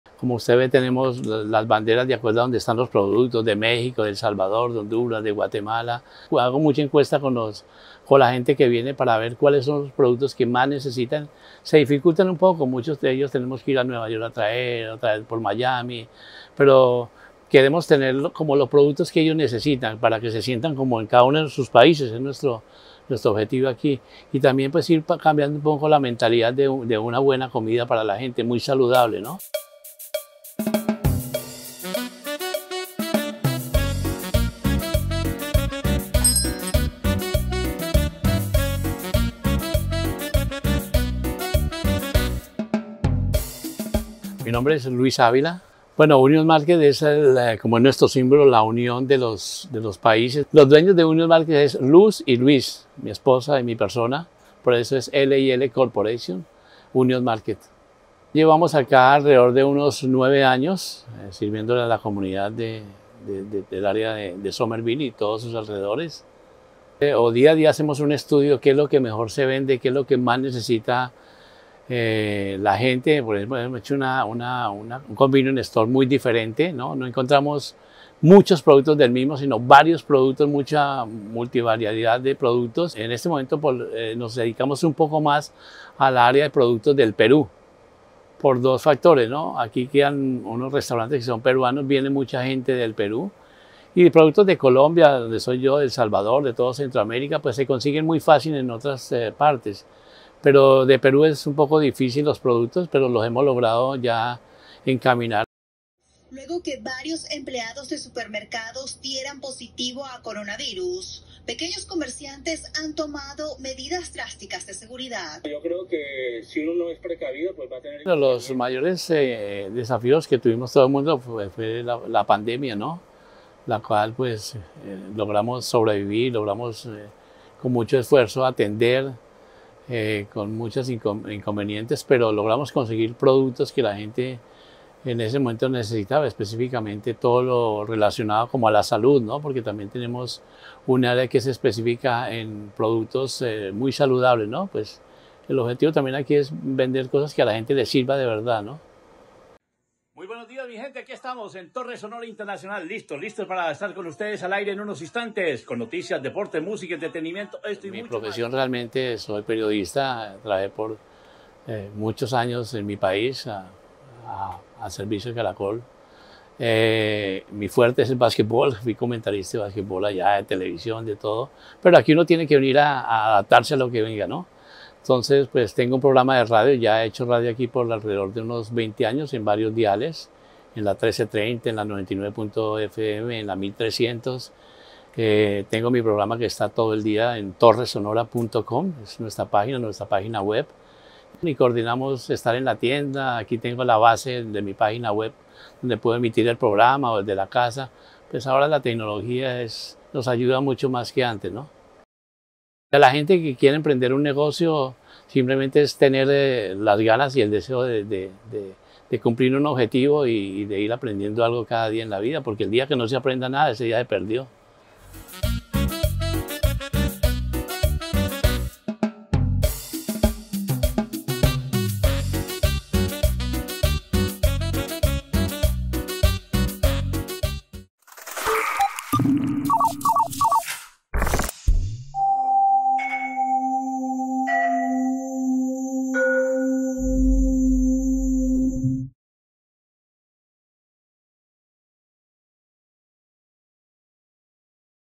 From Talking Basketball to Talking Shop | OFB Ep. 2
In the second episode of Open For Business, Union Market owner Luis Avila tells us about his unique convenience store located in the Union Square neighborhood of Somerville, MA and his career in sports journalism. Special thanks to Michael Robles and the Union Square Main Streets team for their help on this episode.

En el segundo episodio de la serie Open For Business producida por Somerville Media Center, el propietario de Union Market, Luis Ávila, nos habla sobre su exclusiva tienda de conveniencia ubicada en el vecindario Union Square de Somerville, MA y su carrera en el periodismo deportivo. Un agradecimiento especial a Michael Robles y al equipo de Union Square Main Streets por su ayuda en este episodio.